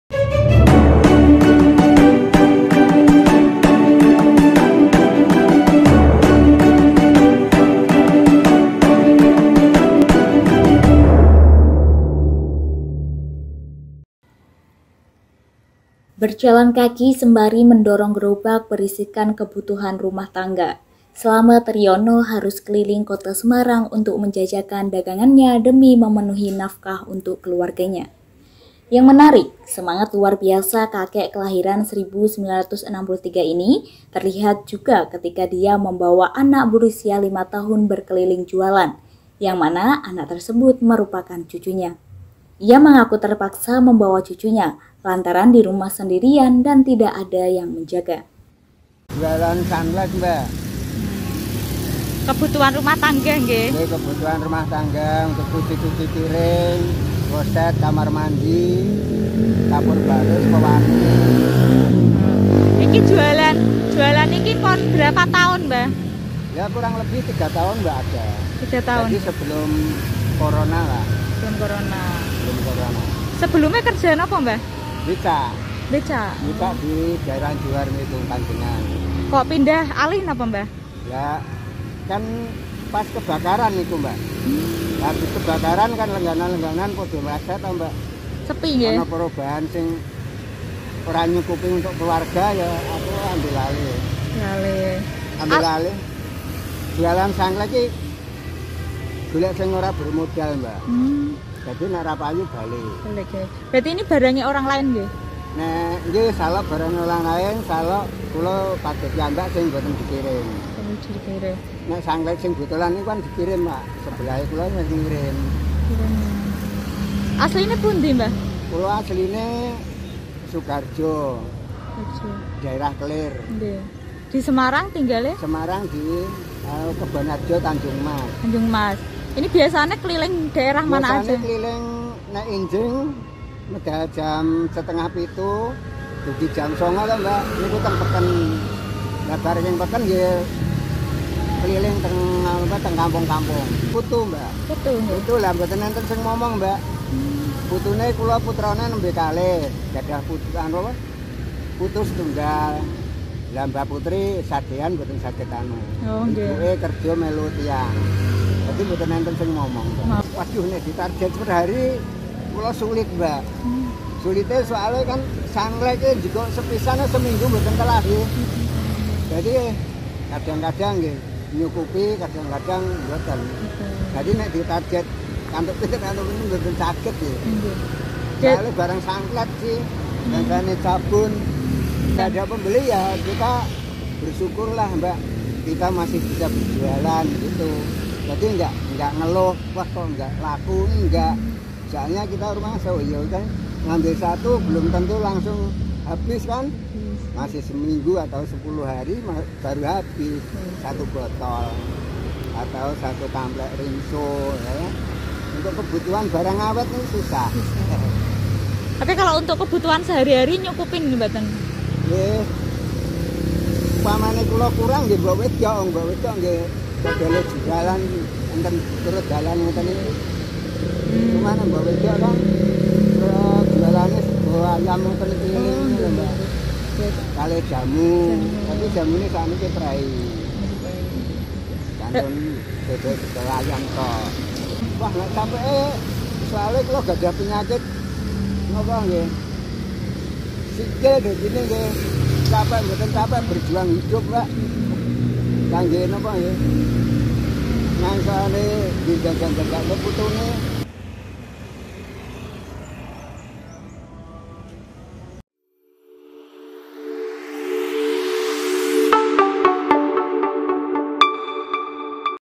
Demi Hidupi Keluarga, Kakek Ini Jualan Keliling Bawa Cucu
Berjalan Kaki sembari mendorong gerobak berisikan kebutuhan rumah tangga, slamet riyono harus keliling kota semarang untuk menjajakan daganganya demi memenuhi nafkah untuk keluarganya.

Yang menarik semangat luar biasa kakek kelahiran 1963 ini terlihat juga ketika dia membawa anak berusia 5 tahun berkeliling jualan, yang mana anak tersebut merupakan cucunya.

Ia mengaku terpaksa membawa cucunya, lantaran dirumah sendirian tidak ada yang menjaga.